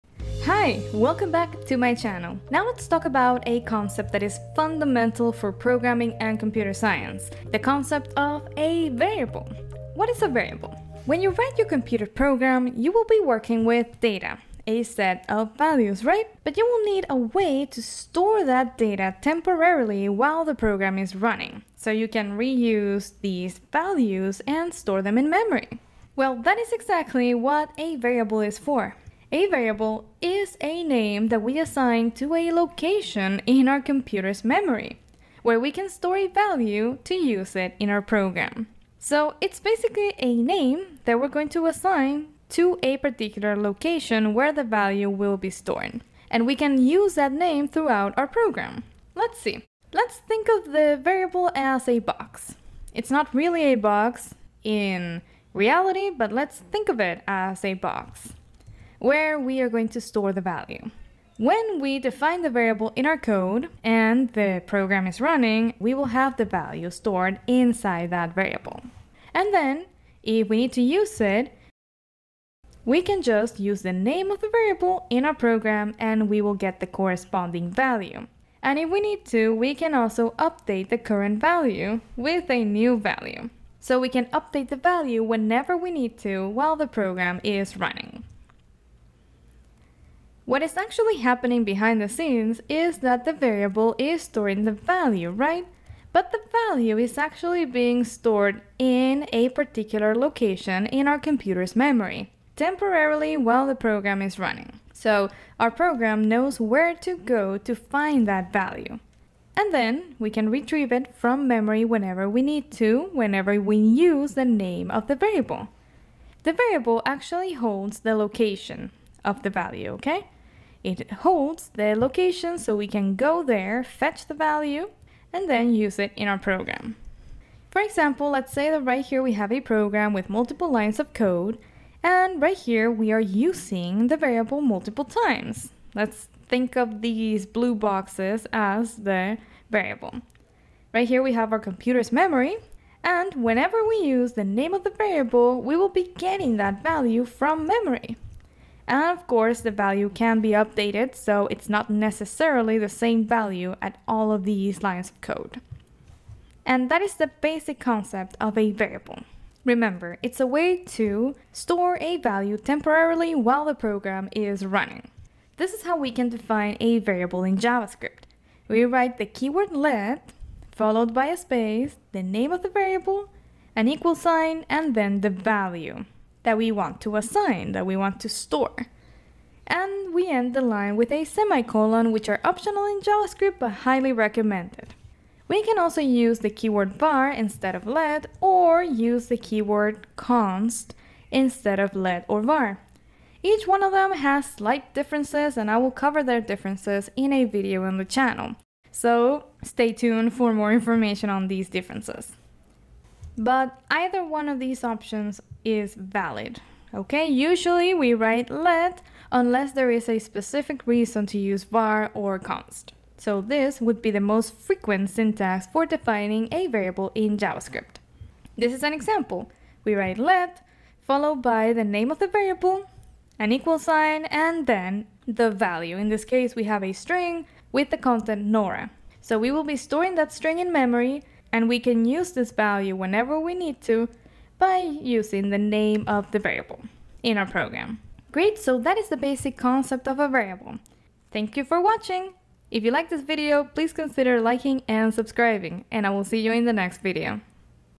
What is a Variable? | Programming Basics
Learn what variables are in programming and why they are very important in just 4 minutes with a JavaScript example.